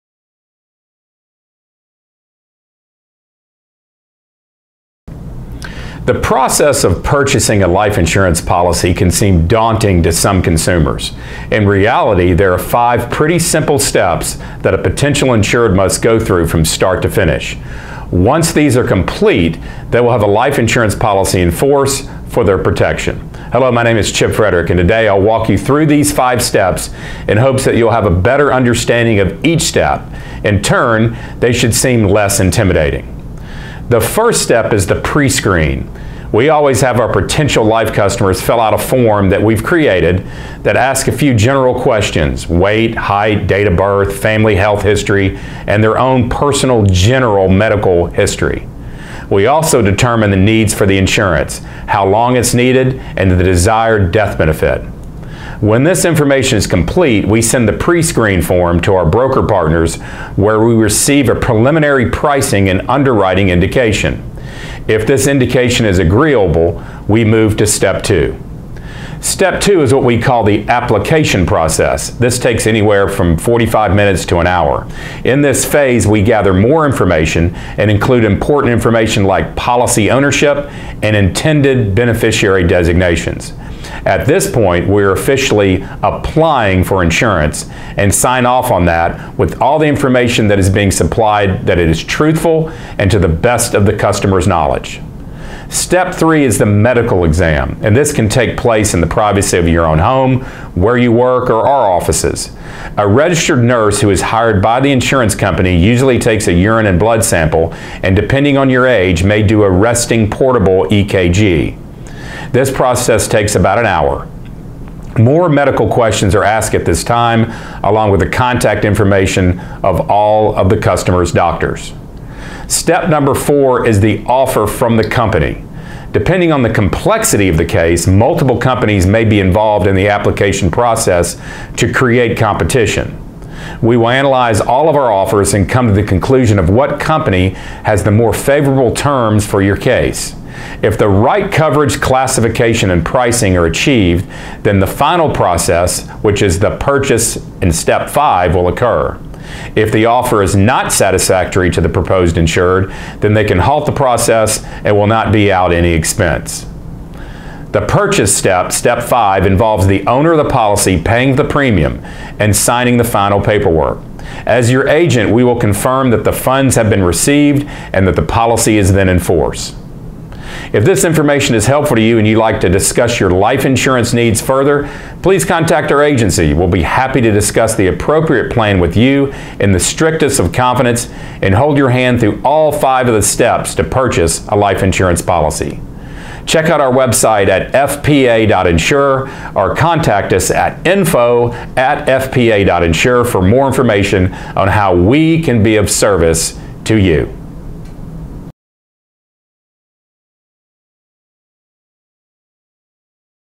The "Steps" To Purchasing a Life Insurance Policy
The process of obtaining a life insurance policy may seem confusing and intimidating but in reality they are pretty simple and straight forward. In this 3 minute video, Chip Fridrich will walk you through the normal steps of the process and what you should expect.

Chip Fridrich is the President of Fridrich, Pinson & Associates Insurance Agency in Nashville, Tennessee. In this short informative video session, he discusses relative topics in the insurance industry in the areas of property, casualty, life, health, and long term disability insurance for today’s consumer.